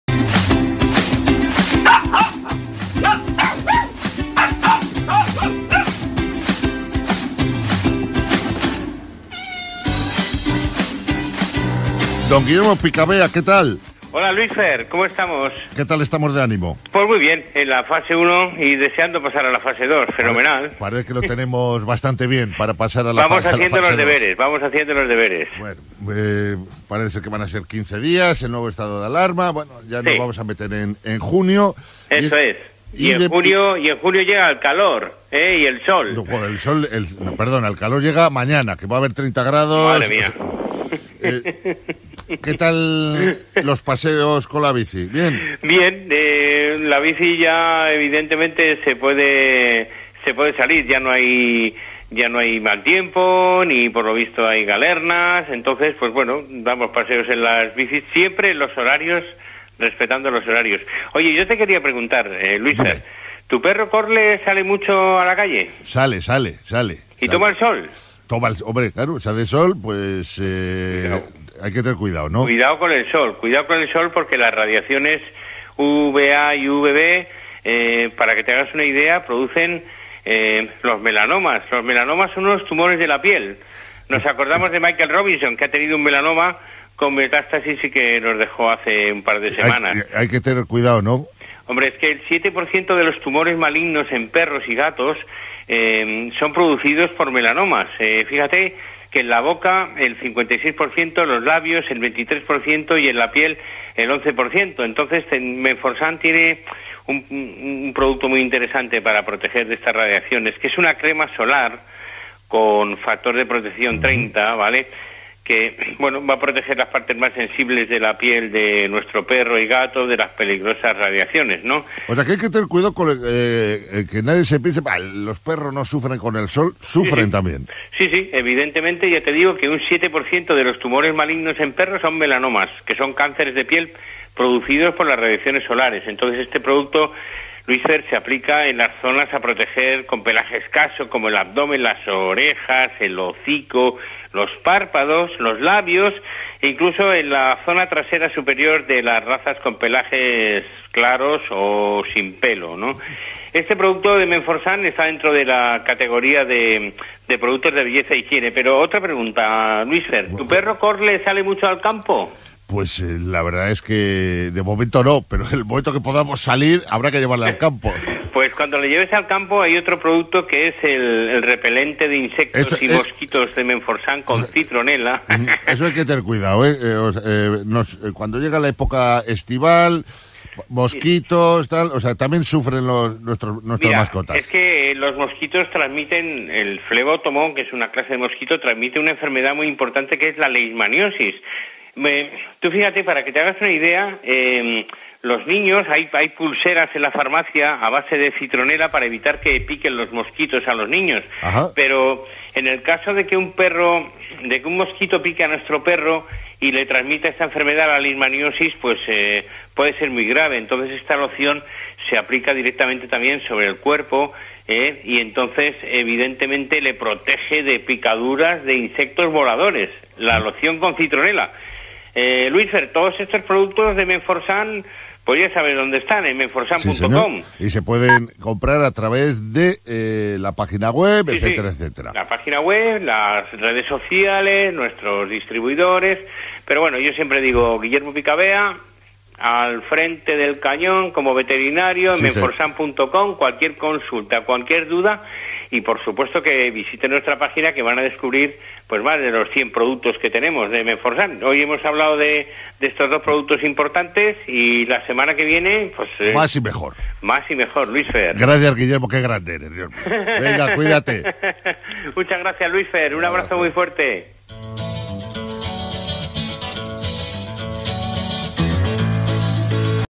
Protege a tu mascota este verano - Onda Cero
Guillermo Picabea, Veterinario y Jefe de Producto de Menforsan en Onda Cero.

La importancia de proteger a tu perro este verano frente a pulgas, insectos y garrapatas pero también frente al sol, que es muy peligroso para la piel de nuestra mascota.